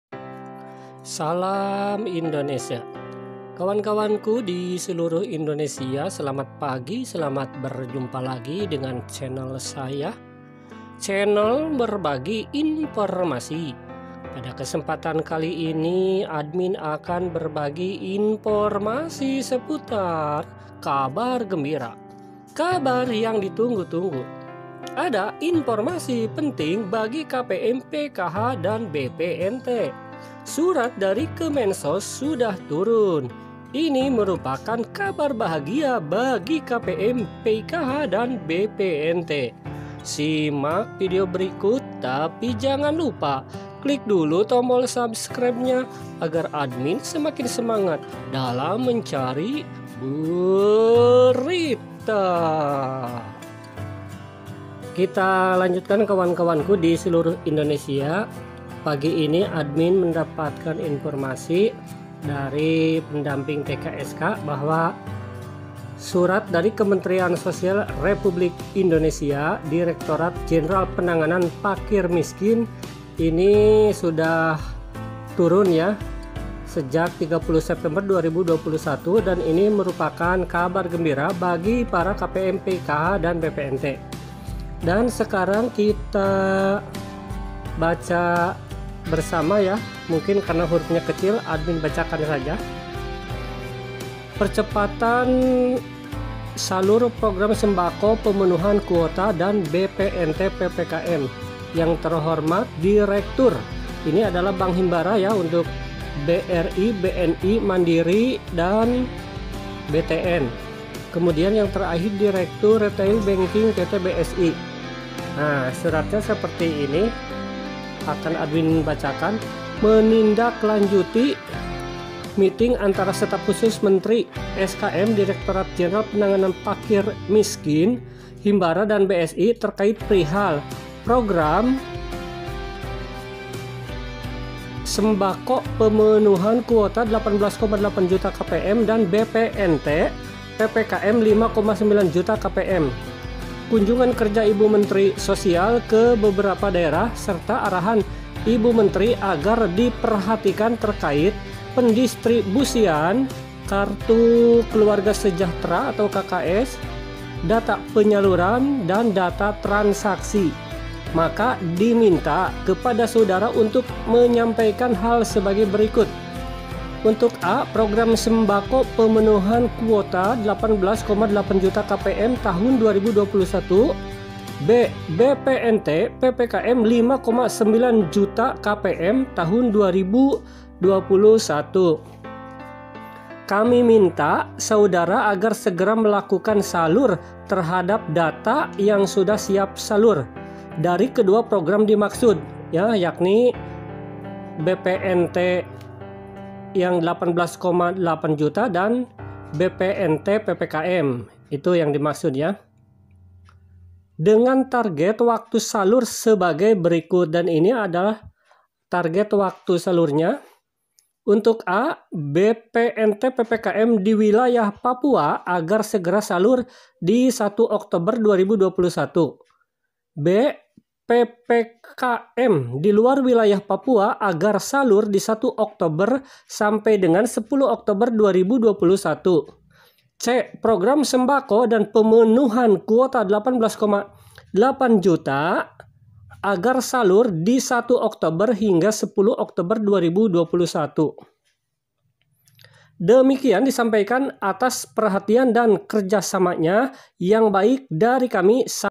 Kabar Gembira Kpm PKH BPNT Surat Kemensos Sudah Terbit
Surat dari kemensos sudah terbit untuk penyaluran bansos Oktober 2021.